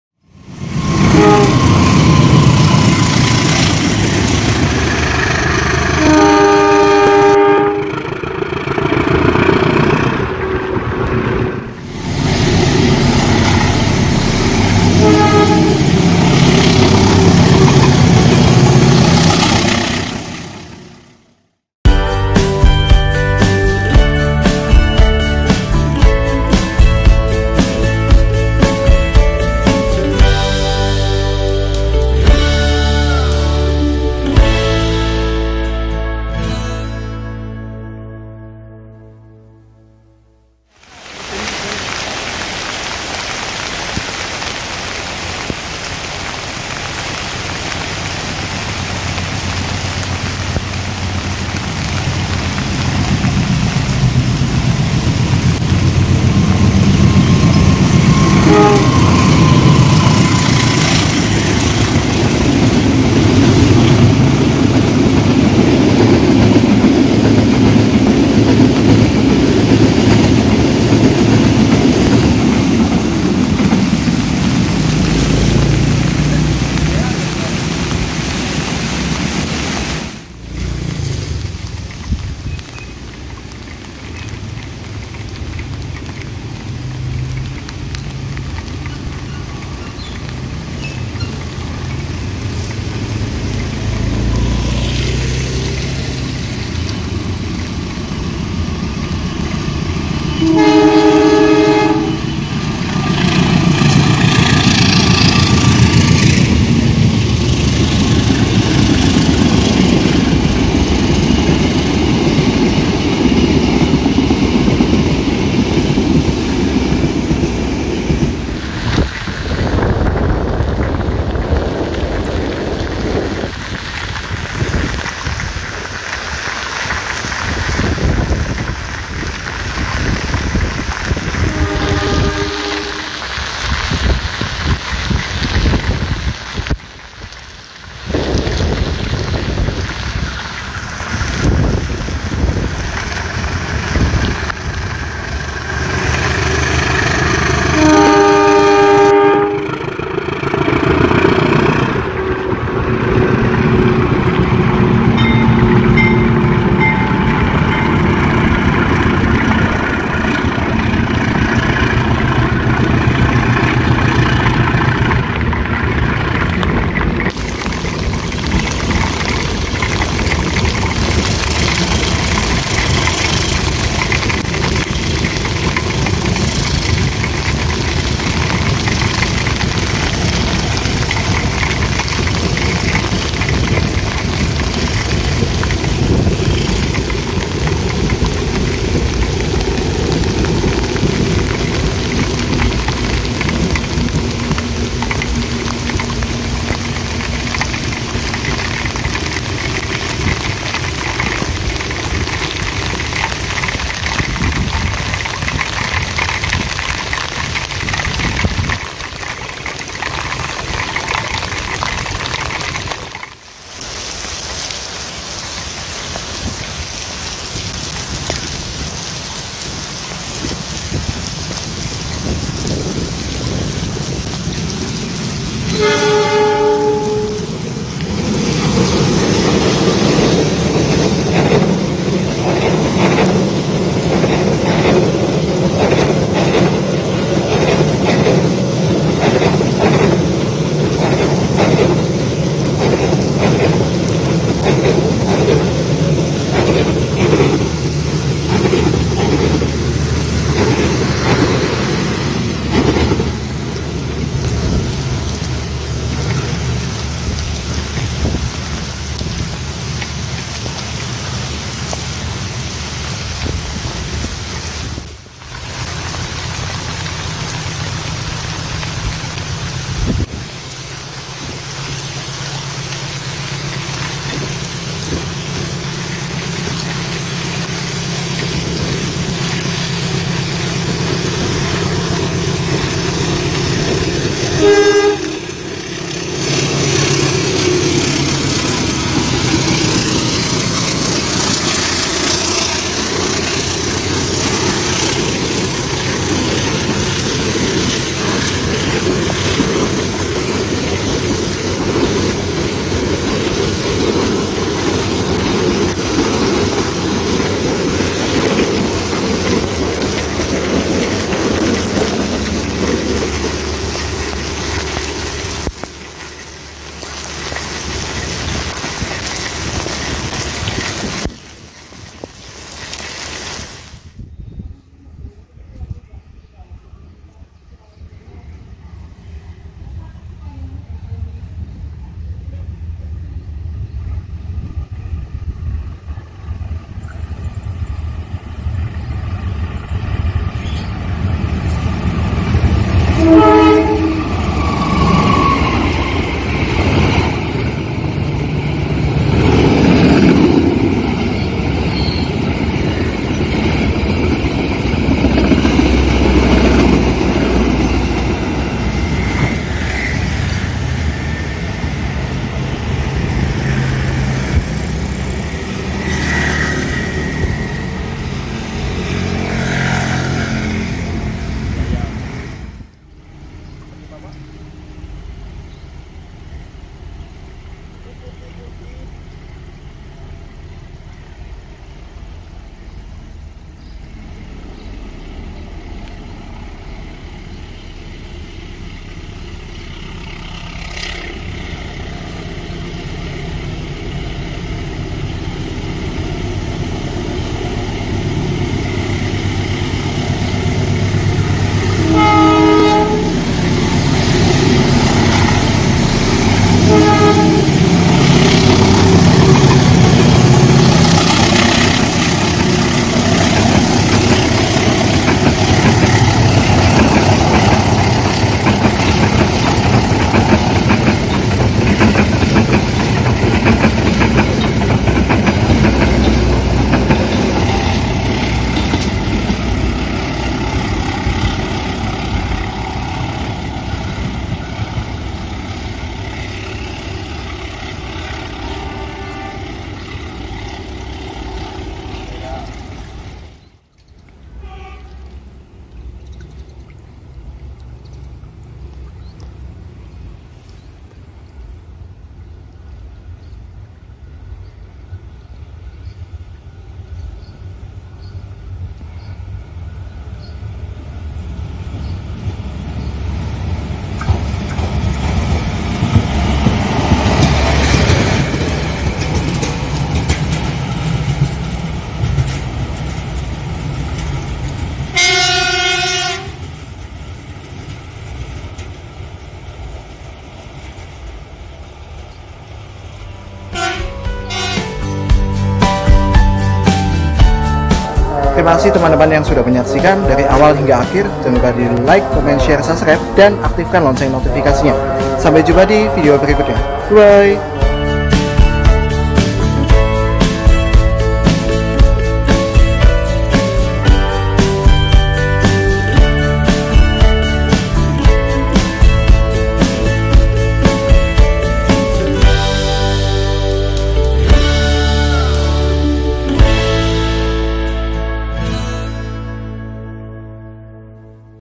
LOKOMOTIF RAILSPRINTER INI MENGELUARKAN API PADA SAAT HUJAN DERAS DI PURWOKERTO ‼️
Pada hari Senin, 25 Maret 2024 terjadi hujan lebat yang mengguyur pada saat siang hari di Kota Purwokerto, tetapi meski tengah terjadi hujan lebat sebuah Lokomotif CC 203 tetap bisa mengeluarkan api atau biasa disebut dengan Kobong. Selain Lokomotif CC 203 yang Kobong, didalam video juga terdapat Kereta Api Sriwijaya loh temen-temen. KA Sriwijaya kan adanya di Divre 3 dan Divre 4 mas dan itu juga jalan Fakultatif ??? Daripada penasaran, Yuk engga usah lama lama, Saksikan video ini dulu dari awal hingga akhir dan Jangan di Skip ya Videonya, Jangan Lupa di Like, Comment, Share, Subscribe, dan Aktifkan Lonceng Notifikasinya.

Chapters :
00:00 Cuplikan Video
1. KA 138 Gajahwong CC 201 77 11 SDT ( Pasar Senen - Lempuyangan ) 00:42
2. KA 79F Manahan CC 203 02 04 MN ( Solo Balapan - Gambir ) 01:21
3. KA 79F Manahan CC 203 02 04 MN Spesial Lokomotif Kobong 🔥🔥🔥 ( Solo Balapan - Gambir ) 02:00
4. KA 10 Argo Dwipangga + Luxury New Generation CC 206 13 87 CPN ( Gambir - Solo Balapan ) 03:35
5. KA 135 Bogowonto CC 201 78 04 YK ( Lempuyangan - Pasar Senen ) 04:26
1. KA 141 Fajar Utama Yogyakarta CC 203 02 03 SDT Spesial Lokomotif Livery WnB ( Tugu Yogyakarta - Pasar Senen ) 05:26
2. KA 141 Fajar Utama Yogyakarta CC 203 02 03 SDT Spesial Lokomotif Livery WnB ( Tugu Yogyakarta - Pasar Senen ) 06:14
3. KLB MTT ( Multi Tie Tamper) Plasser & Theurer SSP-203 & 09-32 CSM memasuki Stasiun Purwokerto 07:24
08:05 Penutupan Video
08:20 Saksikan Juga Video Lainnya✨️